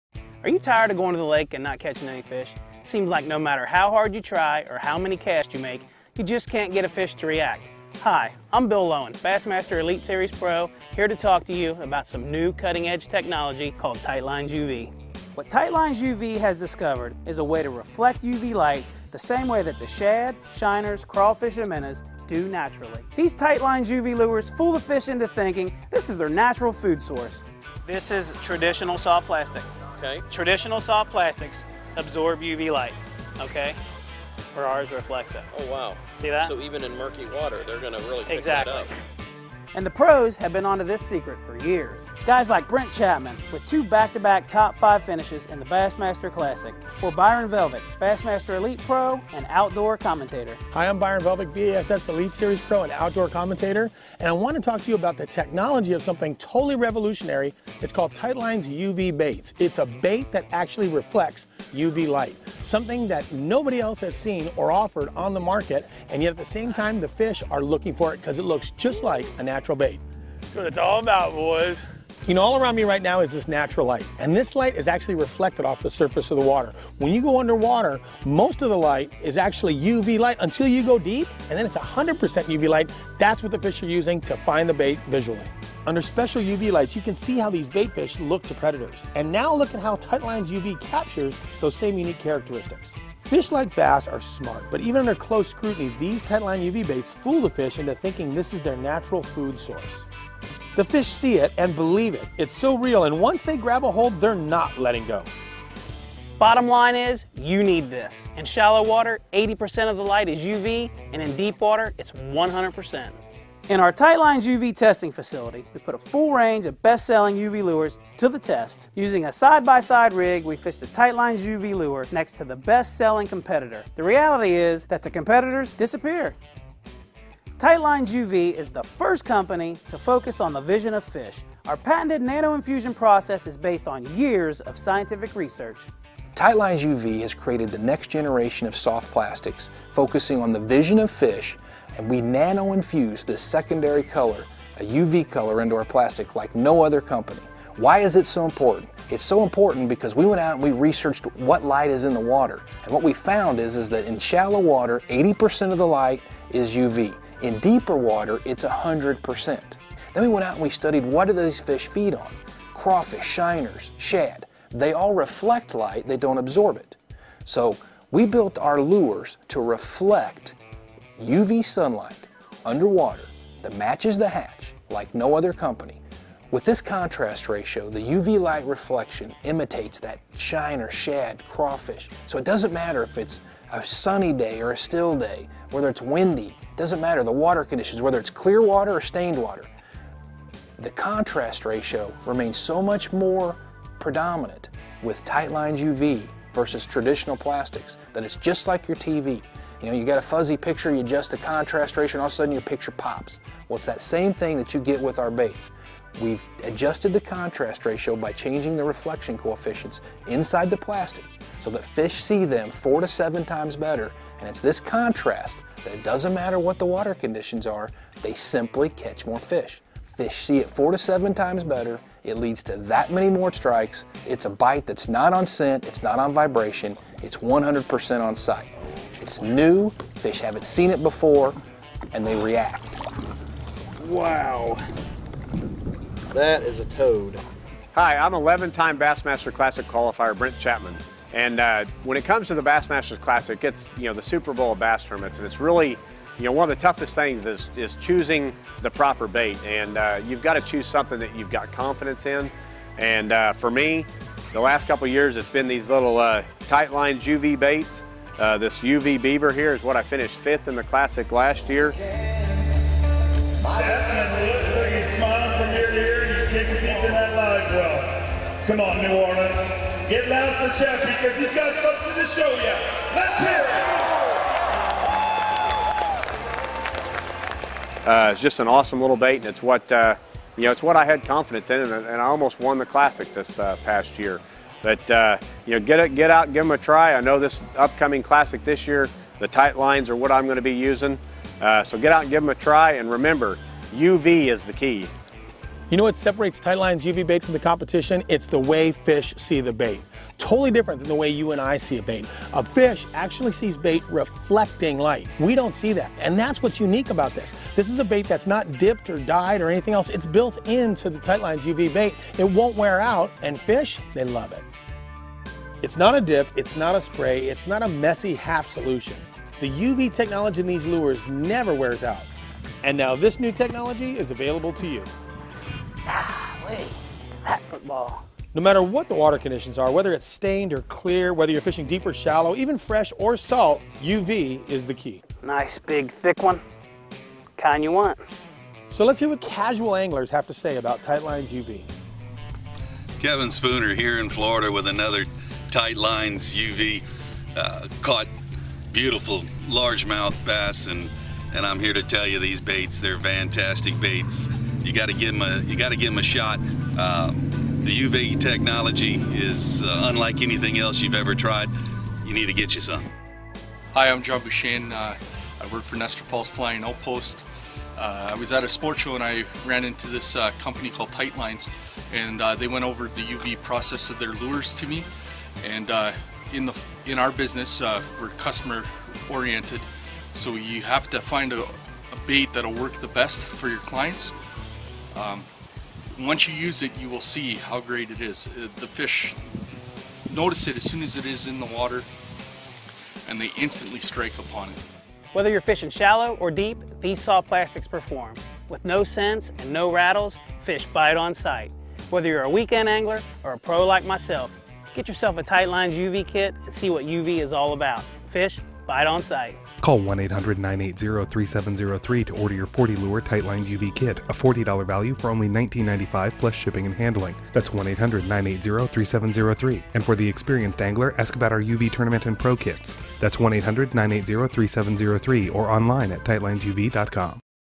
Tightlines UV - The Full Story
The crew at Tightlines UV put together a video to show anglers why the Tightlines UV products are some of the best in the business.

Bad Poncho Outdoors does not own any rights to the Tightlines UV video, the video is owned by Tightlines UV.

If you are interested in the Tightlines UV products check out